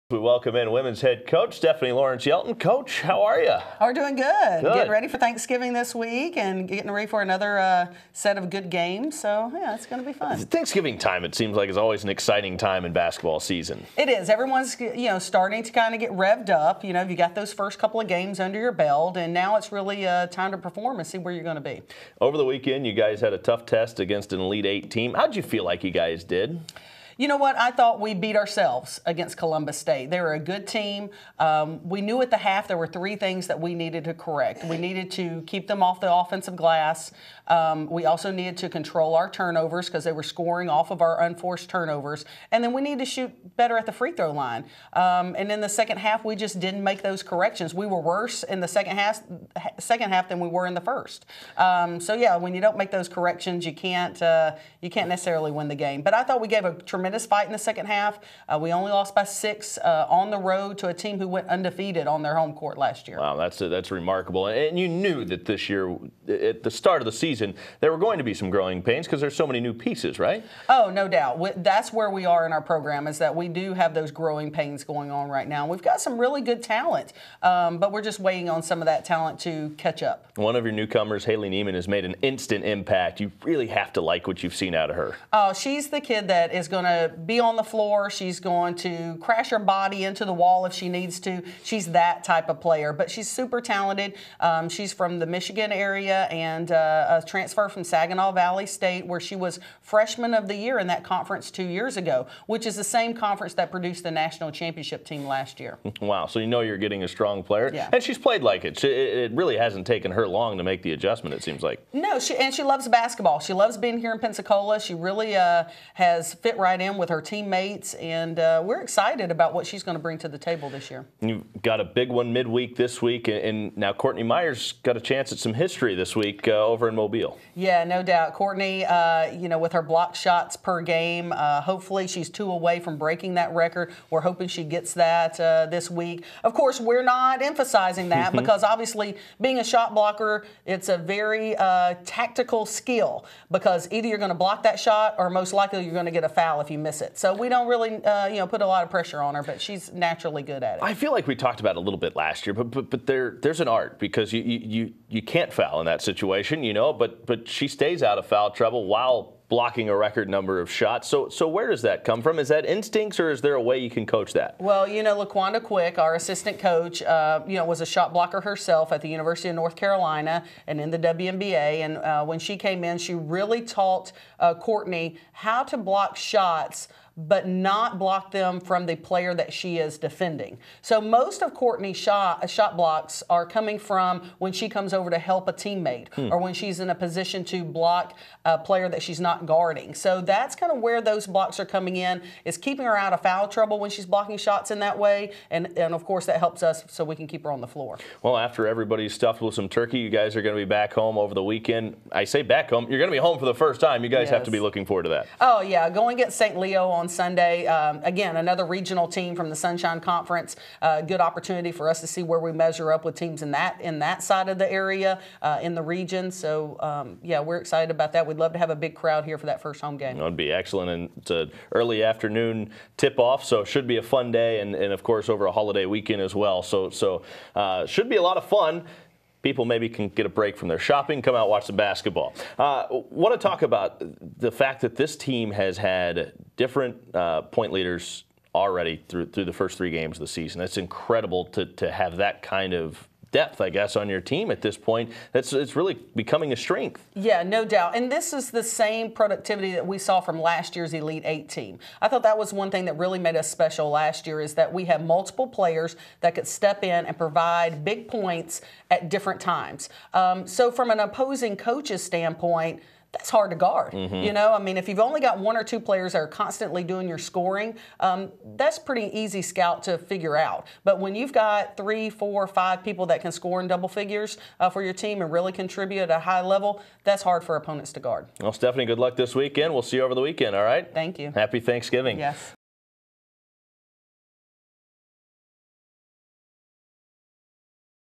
UWFWBB | Weekend Wrapup 11/22
UWFWBB head coach Stephanie Lawrence Yelton sat down this week with Tommy Thrall to discuss the first couple of weeks of the team's season, and preview Sunday's home-opener against Saint Leo.